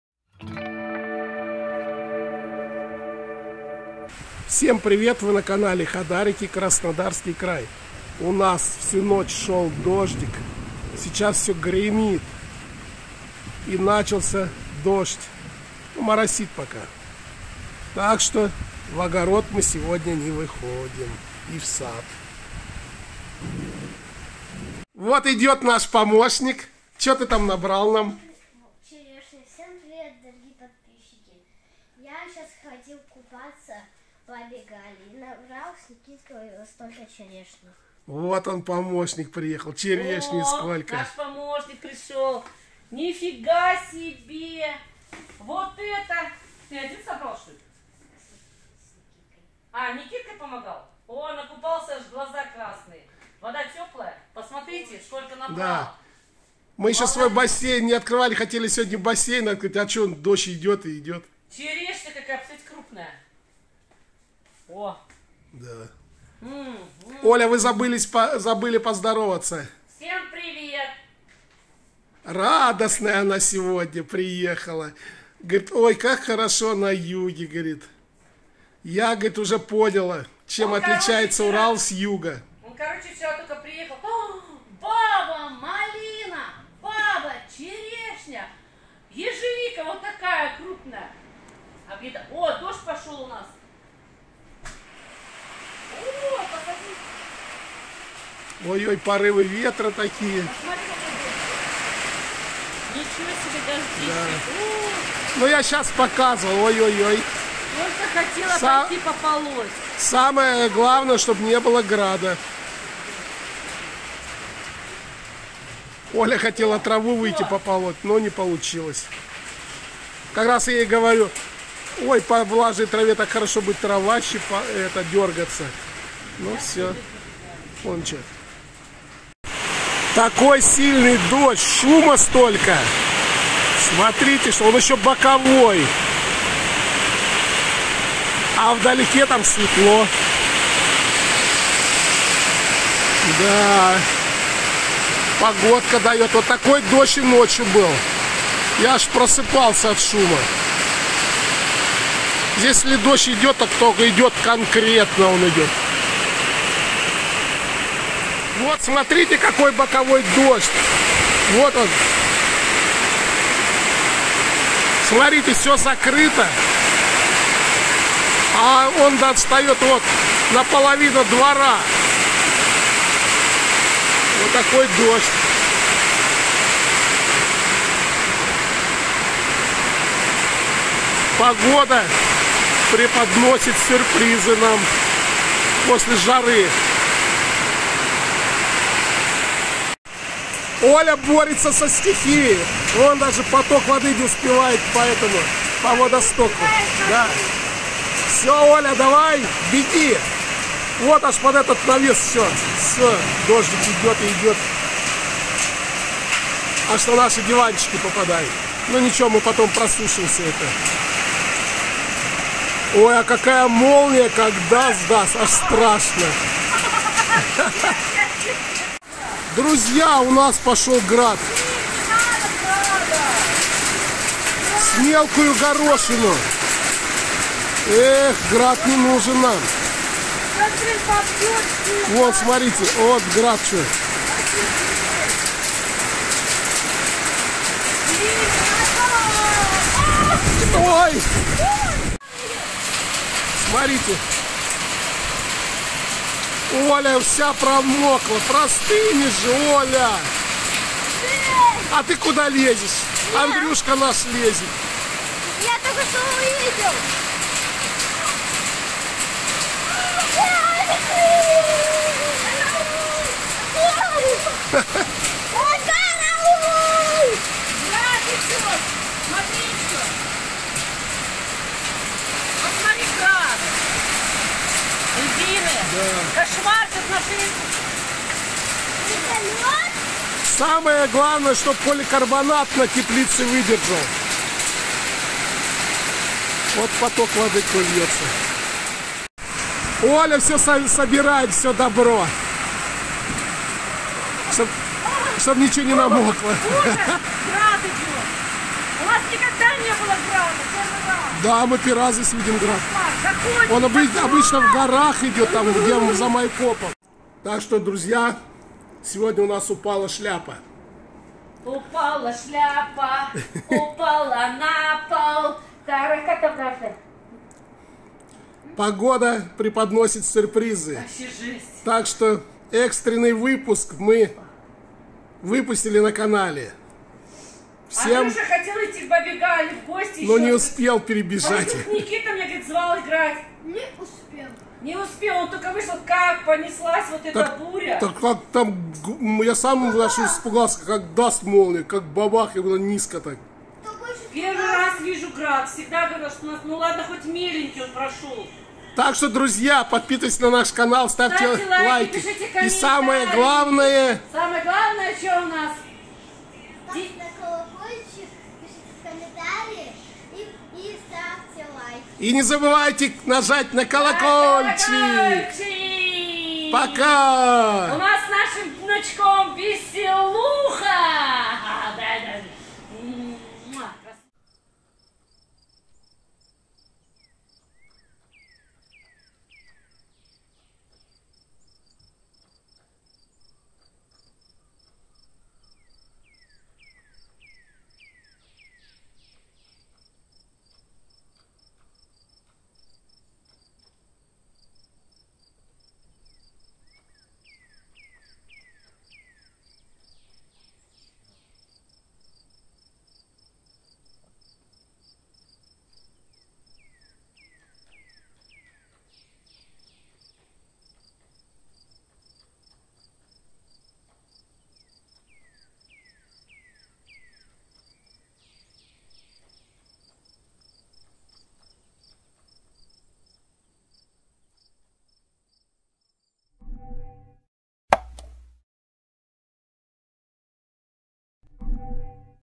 145. Дождь, град, ливень на Кубани.
В Краснодарском крае идут дожди, местами ливни с градом. Обычно град всегда идёт в горах. За три года жизни на Кубани, первый раз мы увидели град у себя в посёлке, град был редкий размером с горошину. Дожди в Краснодарском крае если идут то хорошие из-за этого на юге всё хорошо растёт.

Показываем повседневную жизнь семьи, переехавшие жить на юг с Урала.